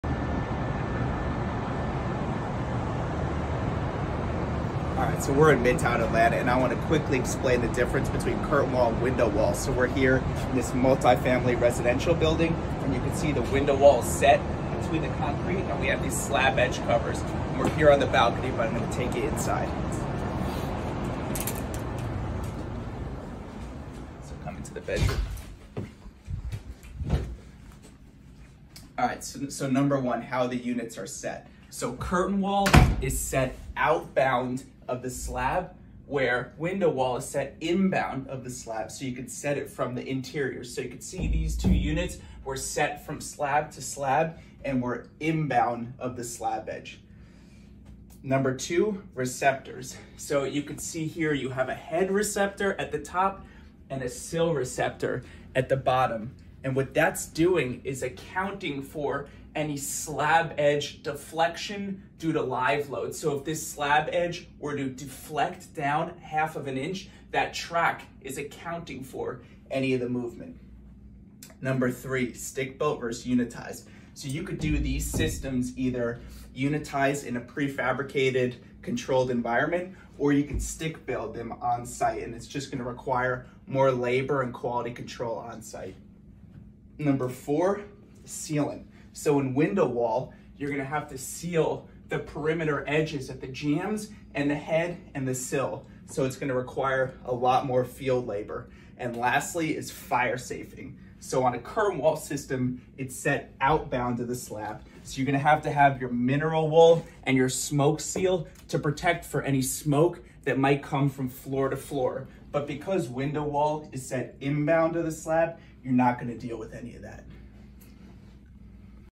Decoding Window Wall vs. Curtain Wall in Midtown Atlanta
We cover 5 principles to start:
◾️Setting conditions - installing from the interior.
◾️Receptors - accounting for movement (deflection) and water management.
◾️Stick built vs. unitized.
◾️Perimeter seals - sealant (wet) versus gasketed (dry) interfaces.
◾️Firesafing - managing smoke at the perimeter slab edge.

Hope you got something out of this one and enjoyed.